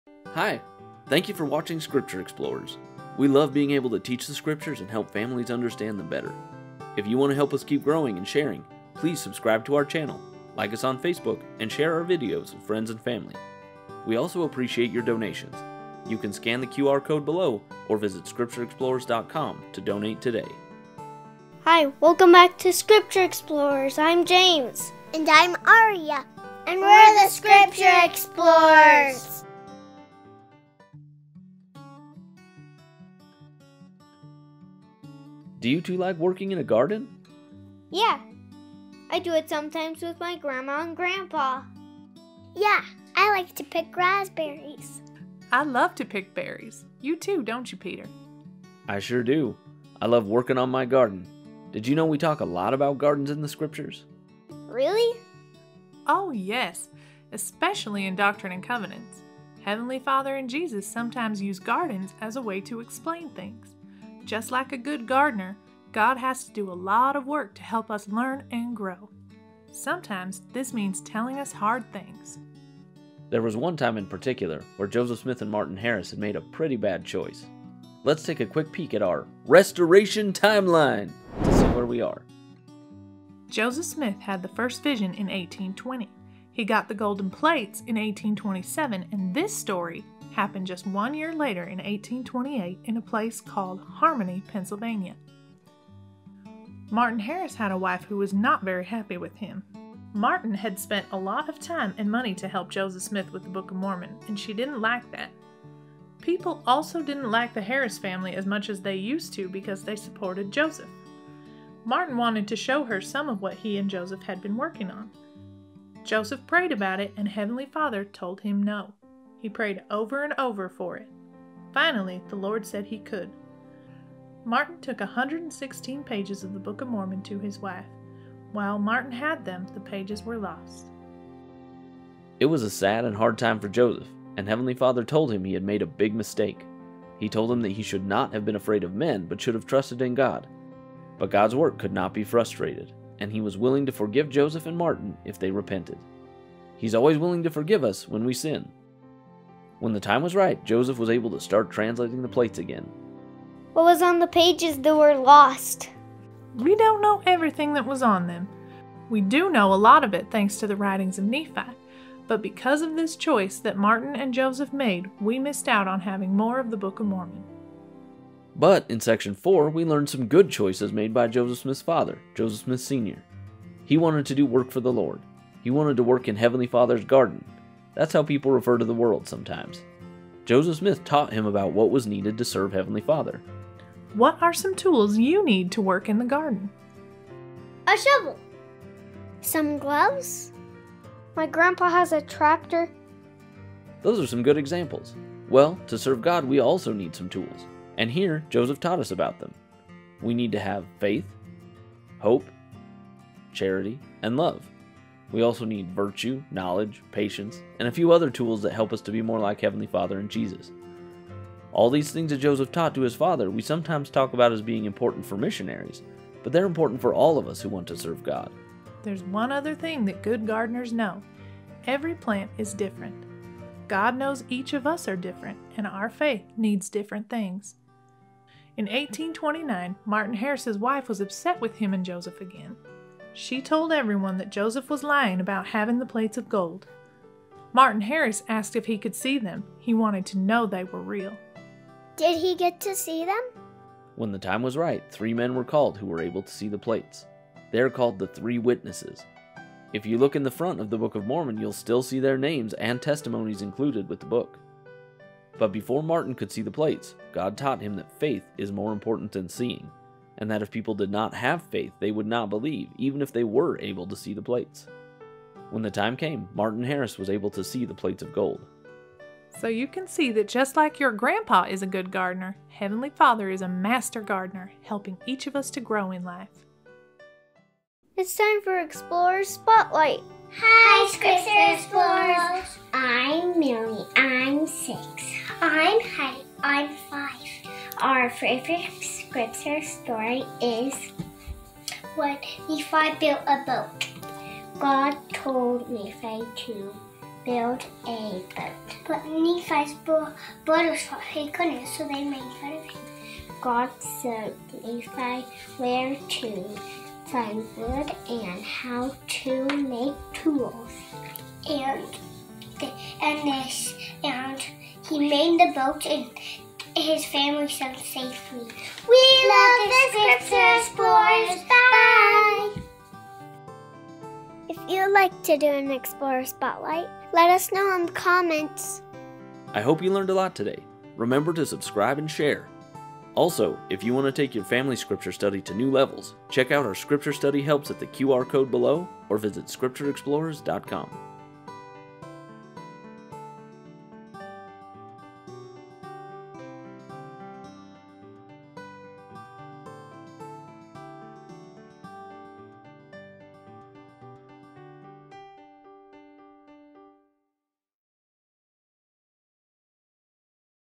D&C 3-5 | The Lost 116 Pages | Come Follow Me 2021 | Doctrine and Covenants
Last time we talked about the first vision, it happened in 1820. Joseph had that vision and then he had to go back to his life. People were mean to him, and he was having a hard time, but then in 1823 something happened.

He was praying one night in his room when all of a sudden an angel appeared to him. But this wasn't just any angel, it was Moroni! He was there to teach young Joseph many things he needed to know about the work he had ahead of him. He also told him about an ancient record that was hidden that Joseph would help to translate. Do you know what that record was? The Book of Mormon!

Moroni told him much more, then he went away. Joseph was thinking about everything he had heard when all of a sudden Moroni appeared again.

Joseph had to go and live his life for a time. He met and married Emma, and he also had a lot more people being mean to him because he still talked about his vision. He went back to the hill every year as Moroni wanted him to. Finally he was able to take the plates in 1827. This was a hard time for him. people tried to steal the plates from him many times but they were kept safe until Joseph didn't need them anymore.

Moroni came and took them away. But while he had them and worked on the translation he met some important friends. One of these was Martin Harris. We'll learn more about him later, but he helped Joseph with the plates in many ways. One of things he  did that was important was to try to get the translation of the book looked at by someone who could say that it was correct. Martin Harris took some of the characters to a man who studied languages, his name was Charles Anthon. Charles Anthon looked at what was written on the paper, and what Joseph Smith had written as the translation. Martin Harris said he wrote a special paper that said it was correct. Then he asked Martin Harris if he could see more of the book. Martin said that he couldn't, because the book was sealed. Charles Anthon then took the paper he had written and ripped it up. This was all part of a prophecy. A long time ago a prophet named Isaiah said that this would happen.